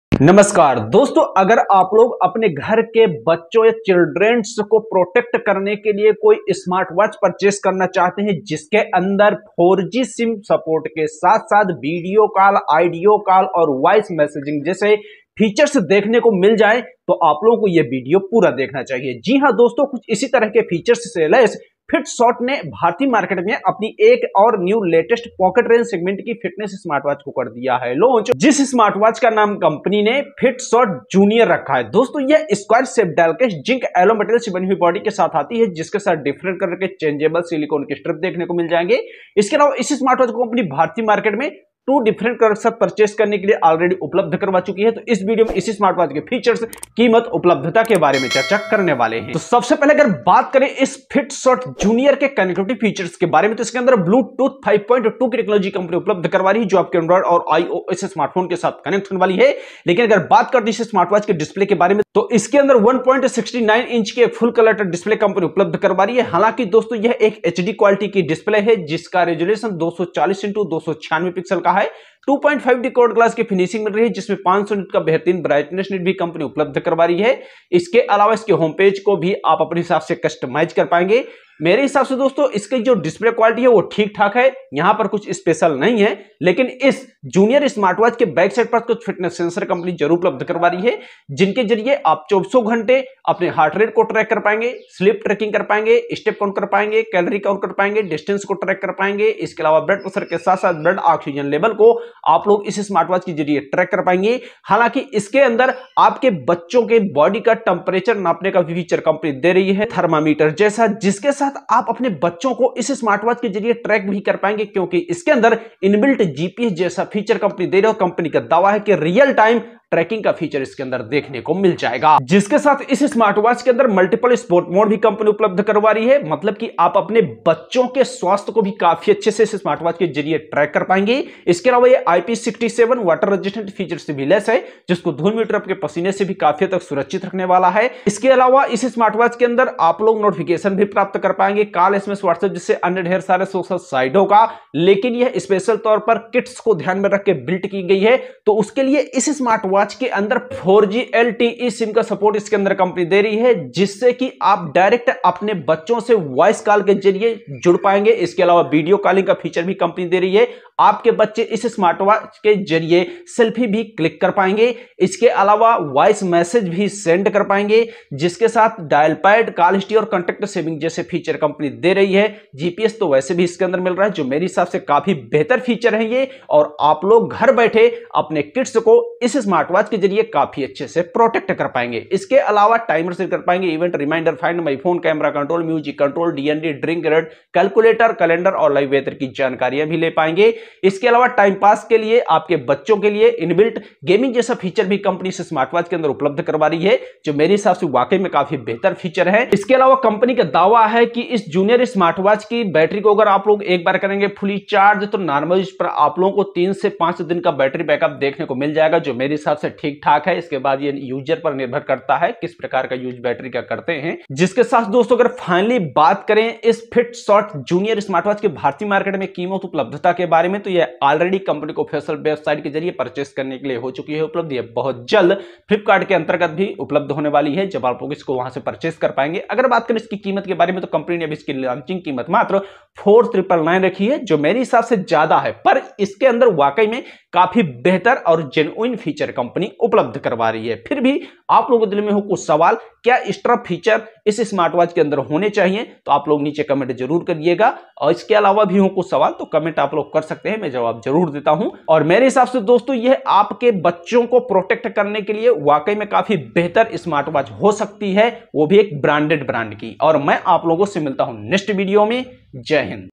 Fitshot Junior Smartwatch | 1.69- HD Display | 4G SIM Support | Video Calling | GPS Tracking | Price
Introducing Fitshot Junior - the ultimate smartwatch for kids! Designed to help parents keep an eye on their little ones while providing them with a fun and interactive experience, Fitshot Junior comes loaded with features such as real-time location tracking, SOS messaging, video calling, and more. With its stylish yet comfortable design and 4G LTE SIM card support, Fitshot Junior is the perfect accessory for your child.
Real-Time Location Tracking: With its built-in GPS and LBS tracking system, Fitshot Junior enables parents to keep an eye on their child's location at all times.
Health Monitoring: Fitshot Junior monitors your child's health by tracking their heart rate, body temperature, sleep quality, and blood pressure.
SOS Messaging & Voice Call: In case of an emergency, your child can use the dedicated SOS button to send an SOS message or make an SOS call to a pre-set number.
Fun & Interactive: Fitshot Junior comes loaded with 7 inbuilt games, a stopwatch, a calculator, and an alarm to keep your child entertained and engaged.
Parental Control: Parents can disable the dial pad and reject unknown calls to keep their child safe and secure. Customizable GPS Tracking Interval: You can set the GPS tracking interval to your preference and get real-time updates on your child's location.
Adjustable Volume & Brightness: You can adjust the volume and brightness of the watch to your liking.
and the the Fitshot Junior Smartwatch Launched In India and available fitshot official website of Rs, 4999/- Only,